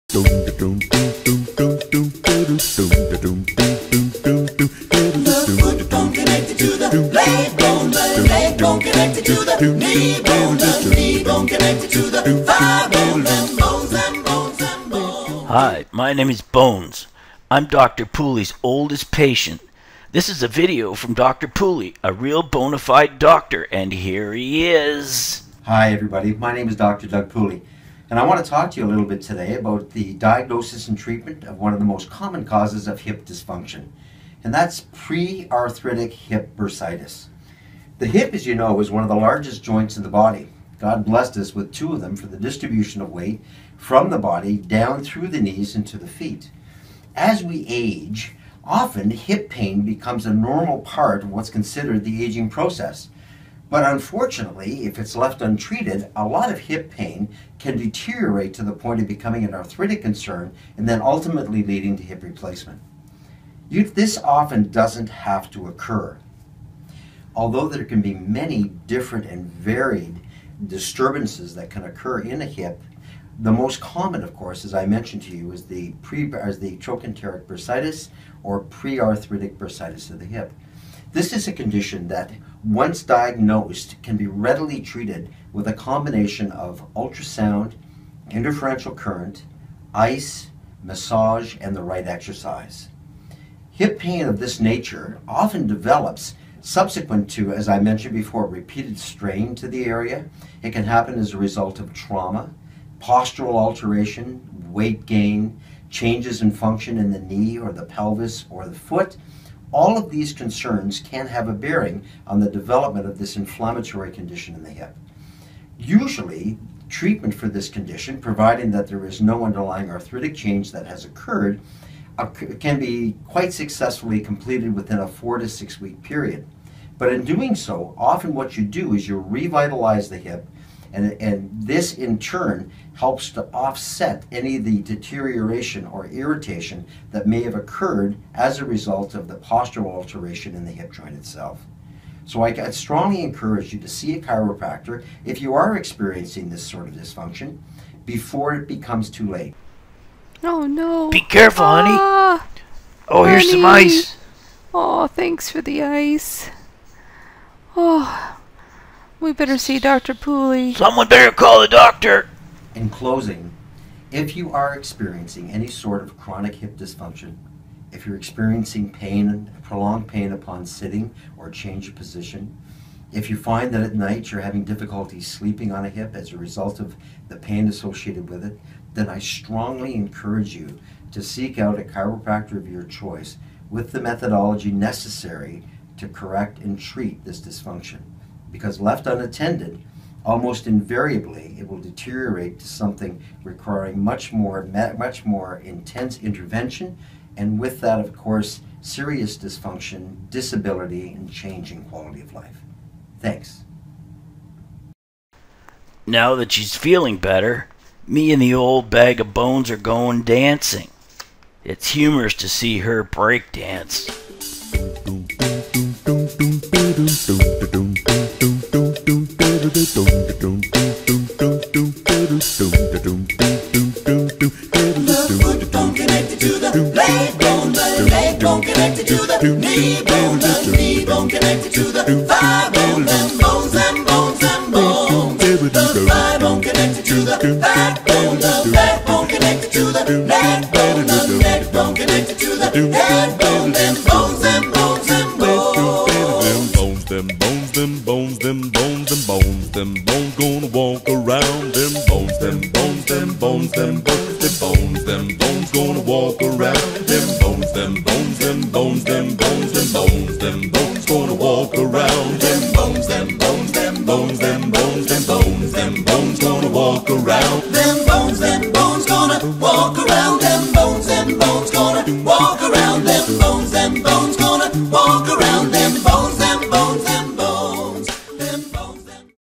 Pre Arthritic Hip Bursitis Dr. Pooley
Dr. Pooley discusses the symptoms and treatment of Pre Arthritic Hip Bursitis.